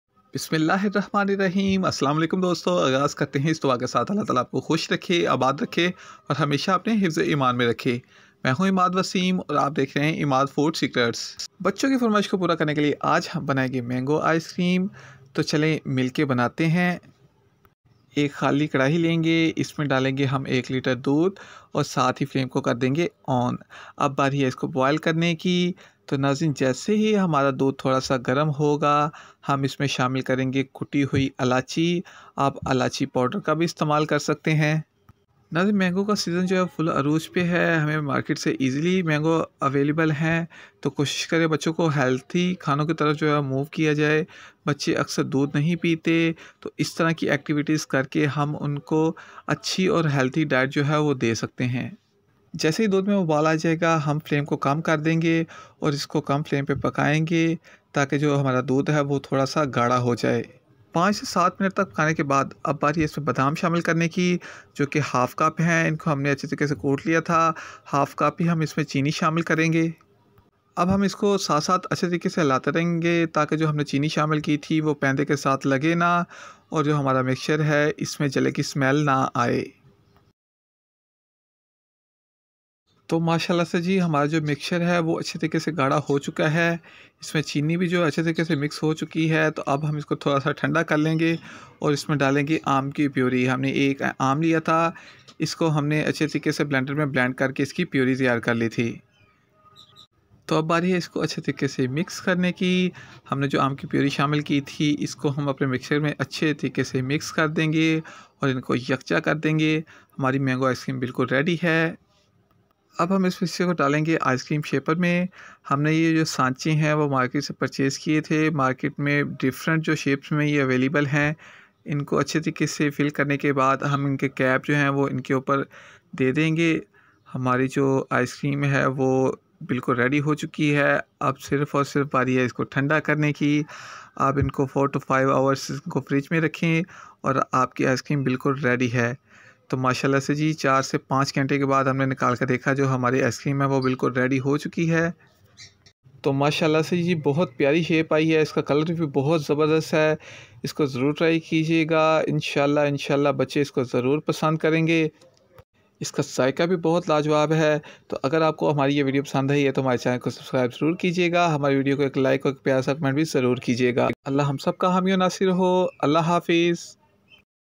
Mango Ice cream | Homemade Ice cream | Imad Food Secrets
Mango ice cream made at home is very easy. just follow few steps and make tasty and delicious ice cream at home.